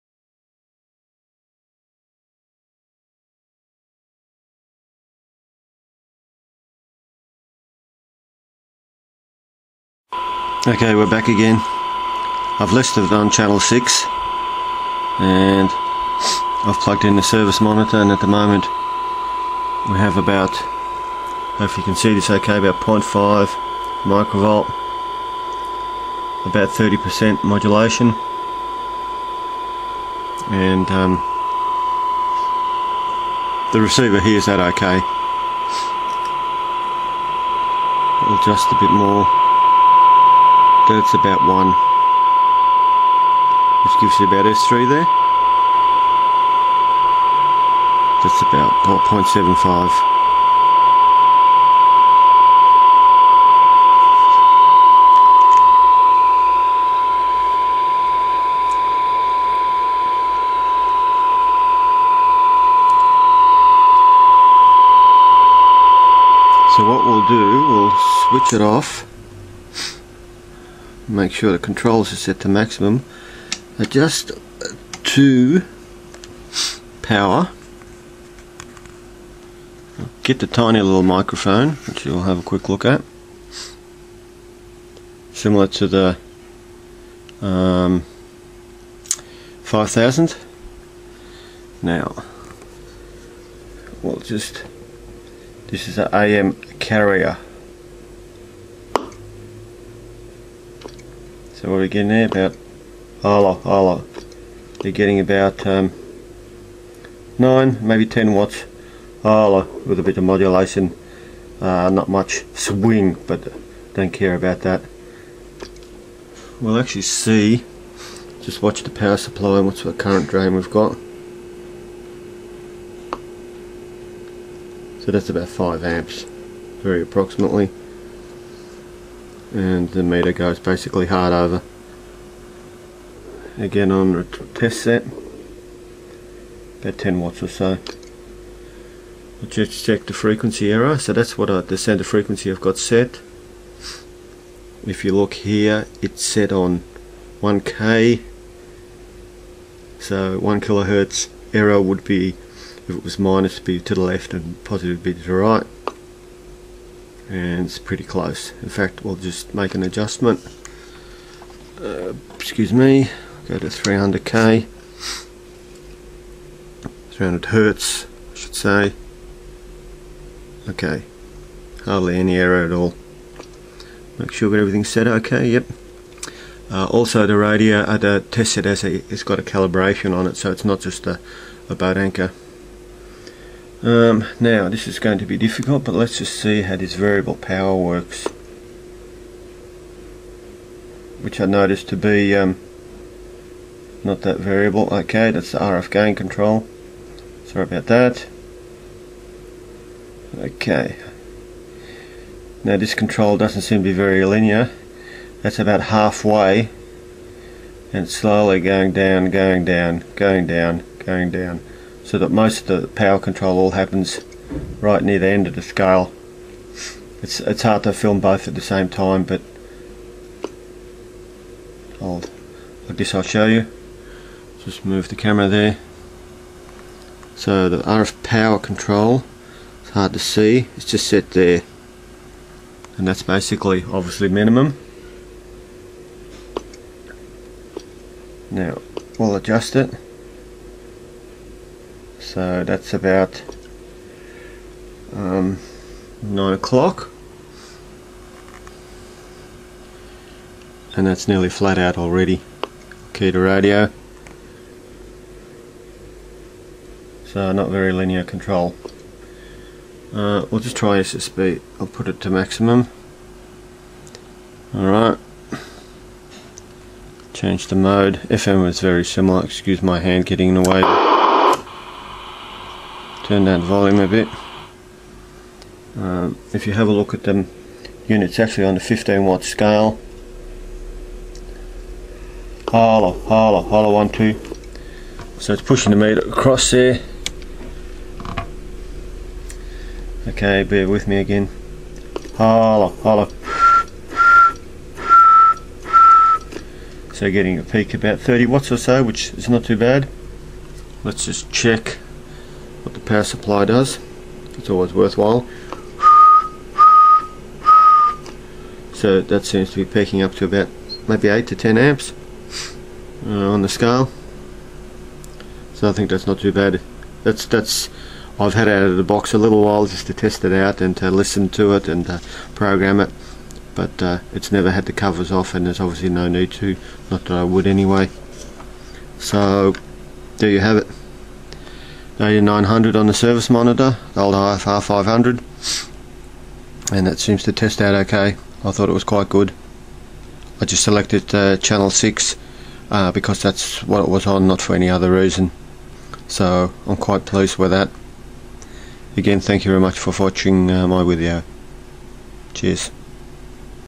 CRE8900 on service monitor.
Hello all, here is my CRE8900 connected to my "borrowed" service monitor, an IFR500 with cal.  I just did some very basic tests on the transmitter and receiver.  All seemed well.  I guess you could peak it up but I think there's no need too.  The radio seems to work well so far.